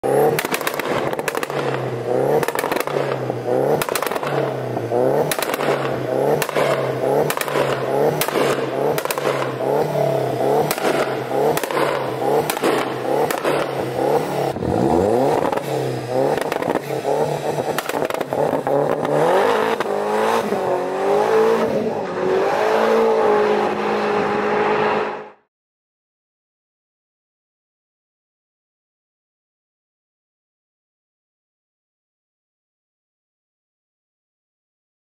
300Bhp Cupra R Crazy Launch Control
300Bhp Cupra R Crazy Launch Control

todays video we take a look back at scott shores 300bhp cupra r and its crazy launch control and exhaust sounds. the car is also on airride with a load of modifications take a look at our full video we done on his car click the link to check that out.